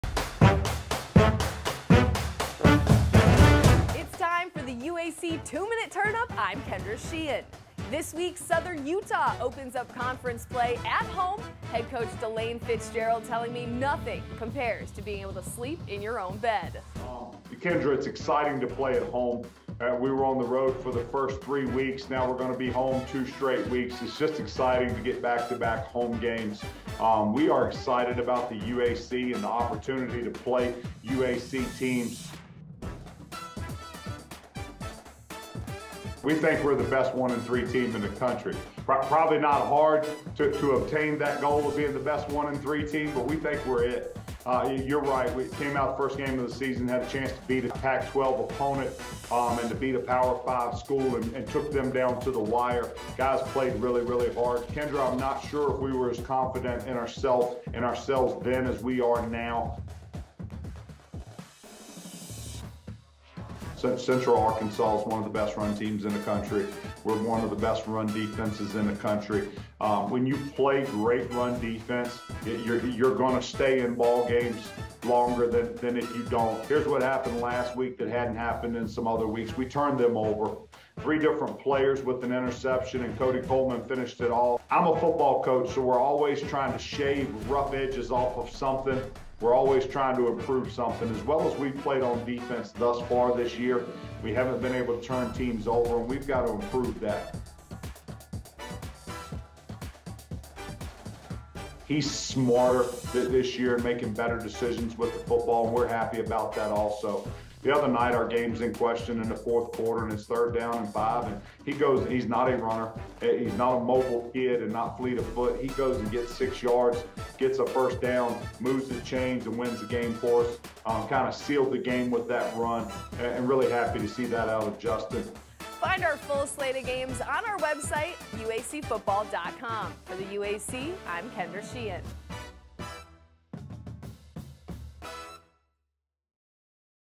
UAC 2-Minute Turn Up: Southern Utah Thunderbirds
Kendra Sheehan visits with Southern Utah Head Coach DeLane Fitzgerald to discuss the TBirds first victory and also their UAC Football opener against Central Arkansas.